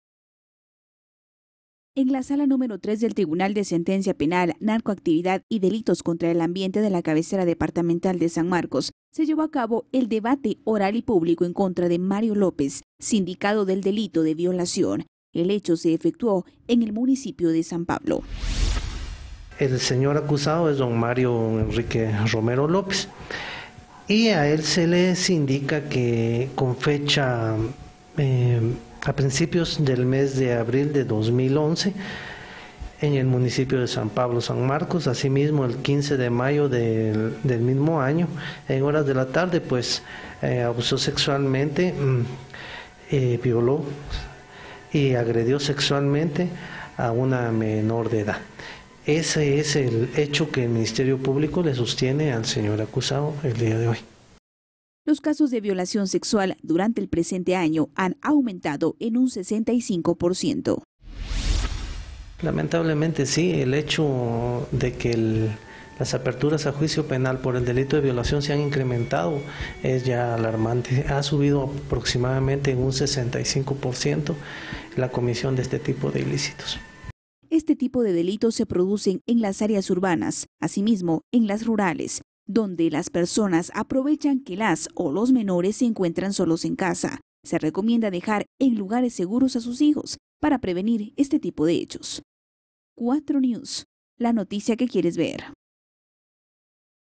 AUMENTAN CASOS DE VIOLACION SEXUAL
Dio inicio el debate oral y público, en el caso de un presunto violador, autoridades han indicado que el hecho se dio en el municipio de San Pablo.